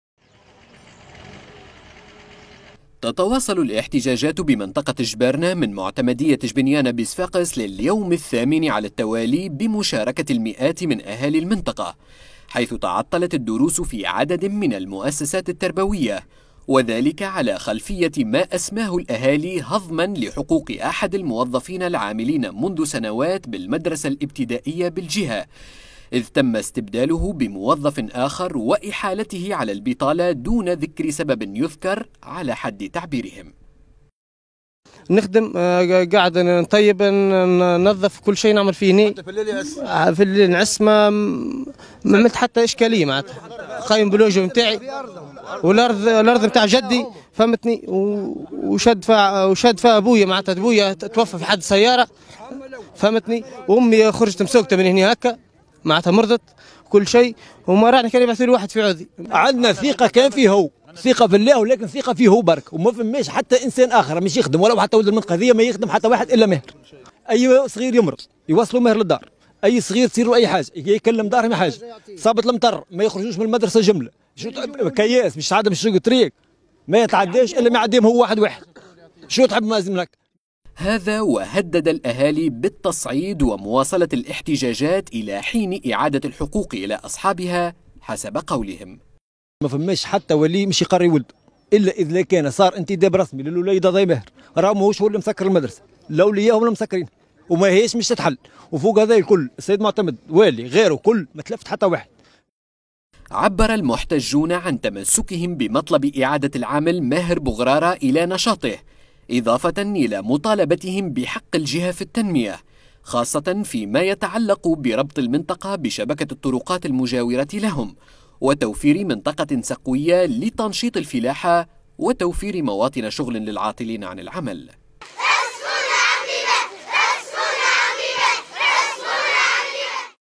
صفاقس -عمادة الجبارنة .. احتجاجات منددة بعزل أحد العمال بالمدرسة الابتدائية ومطالبة بالتنمية
تصوير: حسام الزواري   مونتاج : صلاح الدين محيمدة   تقرير : بسام بن ضو